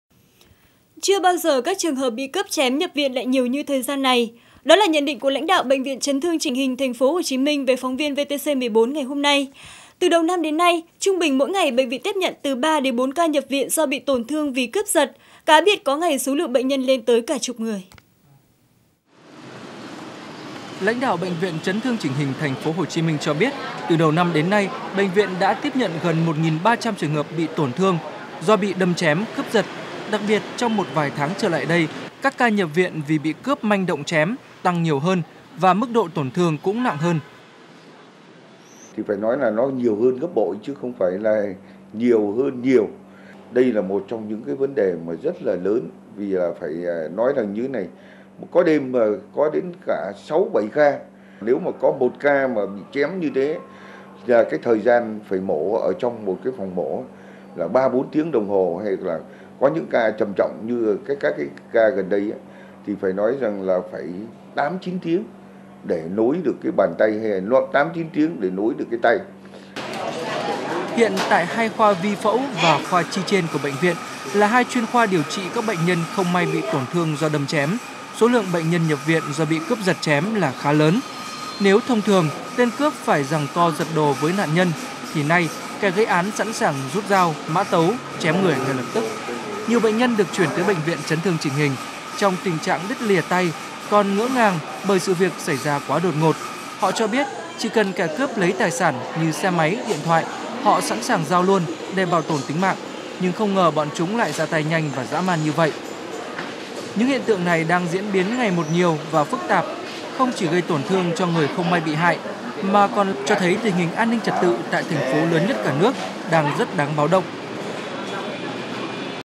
VTC14_Tp.HCM: Nhập viện vì cướp chém tăng_19.12.2012
Tp.HCM: Số lượng bệnh nhân nhập viện vì bị cướp chém tăng mạnh_ngày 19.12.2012.